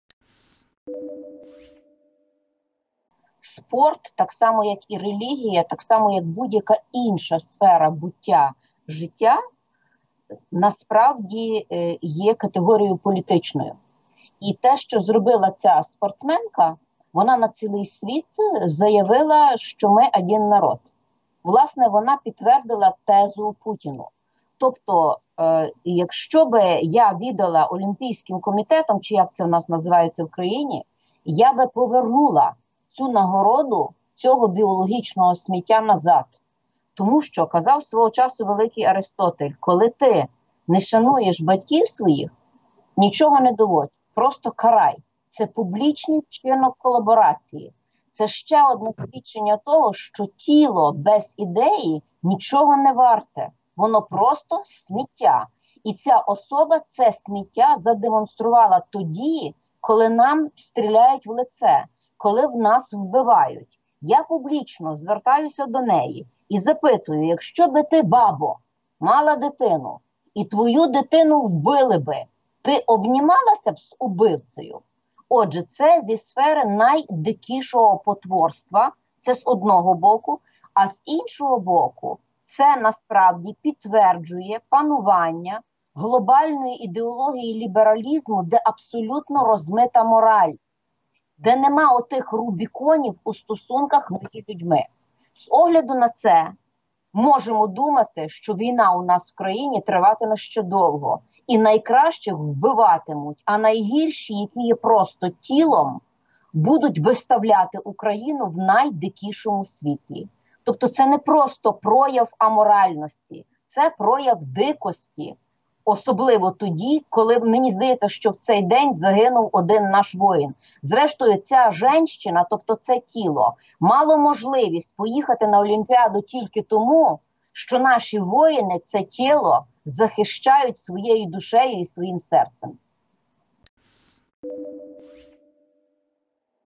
Ірина Фаріон жорстко звернулася до української спортсменки, яка зробила фото з росіянкою
❗️Ірина Фаріон жорстко звернулася до української спортсменки, яка зробила фото з росіянкою після перемоги на Олімпіаді: «Якщо б ти мала дитину і її вбили, ти б обіймалася з убивцею?»